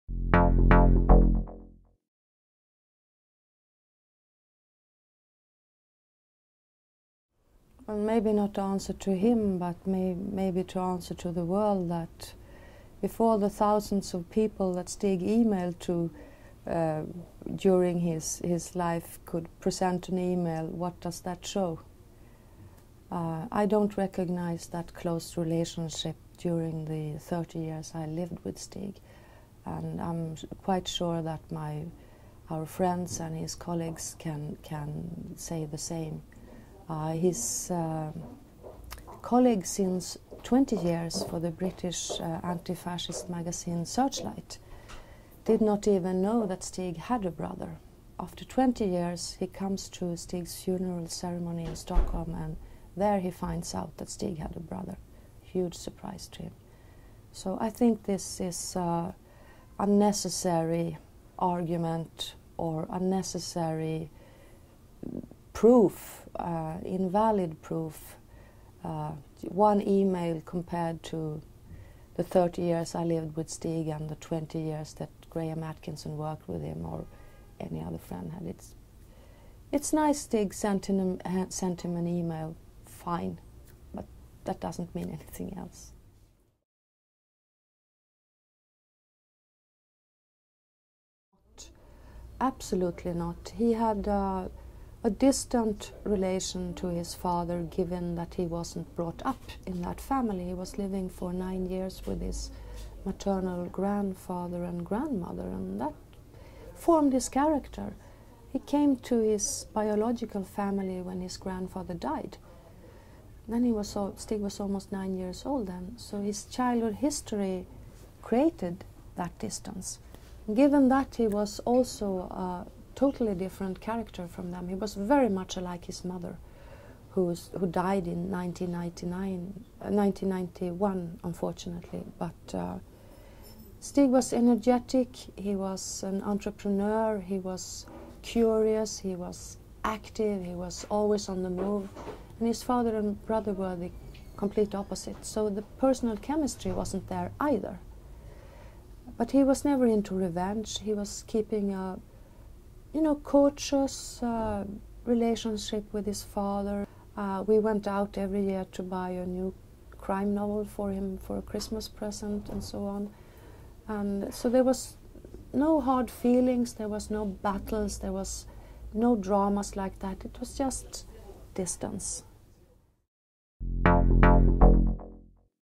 Eva Gabrielsson répond au frère de Stieg Larsson
Eva Gabrielson à découvert la publication de cette mail échangé entre Stieg et son frère, quelques secondes avant l'entretien. Pour la veuve, le créateur de Millenium n'a jamais été proche des siens et ce mail ne prouve rien.